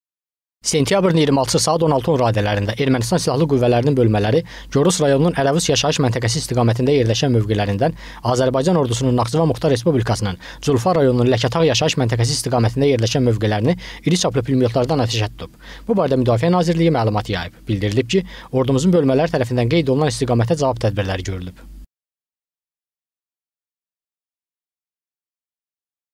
MN: Naxçıvan istiqamətində mövqelərimiz atəşə tutulub – APA TV
Sentyabrın 26-sı saat 16:10 radələrində Ermənistan silahlı qüvvələrinin bölmələri Gorus rayonunun Ərəvus yaşayış məntəqəsi istiqamətində yerləşən mövqelərindən Azərbaycan Ordusunun Naxçıvan Muxtar Respublikasının Culfa rayonunun Ləkətağ yaşayış məntəqəsi istiqamətində yerləşən mövqelərini iri çaplı pulemyotlardan atəşə tutub.
APA xəbər verir ki, bu barədə Müdafiə Nazirliyi məlumat yayıb.

Bildirilib ki, ordumuzun bölmələri tərəfindən qeyd olunan istiqamətdə cavab tədbirləri görülüb.